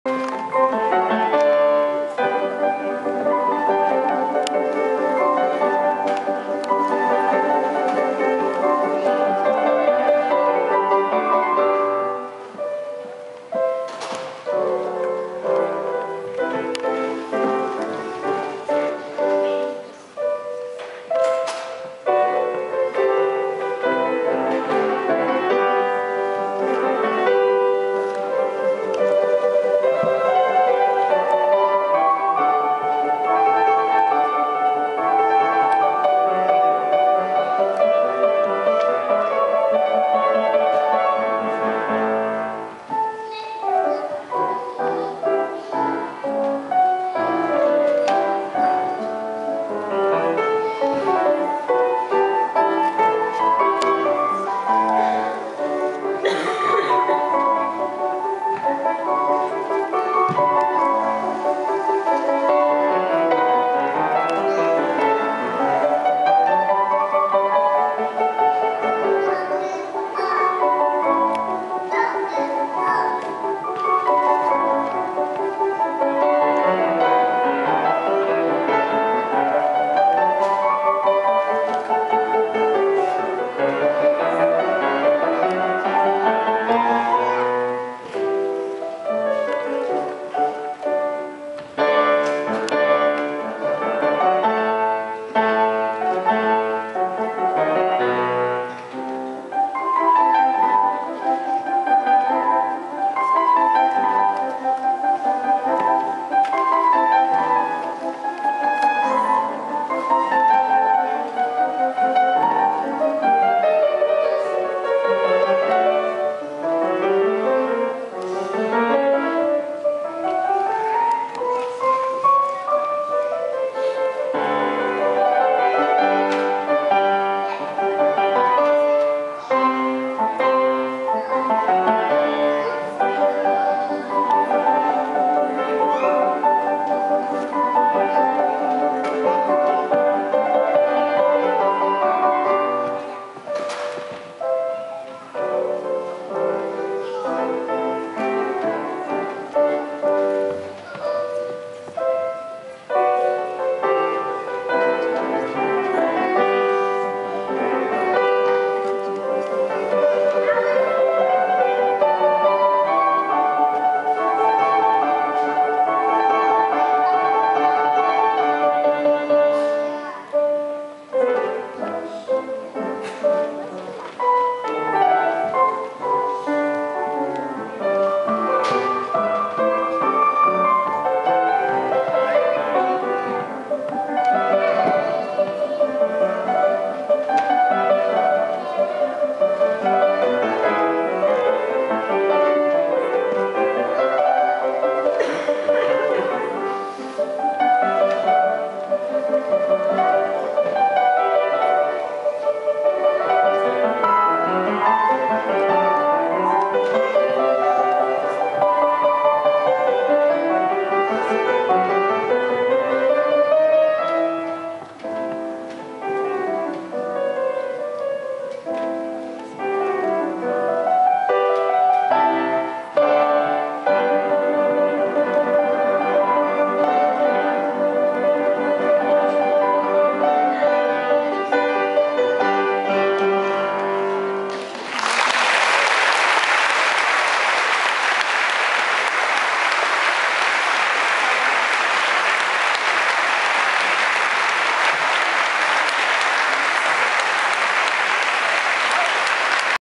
children are playing piano
Conservatory of Music in Victoria, Canada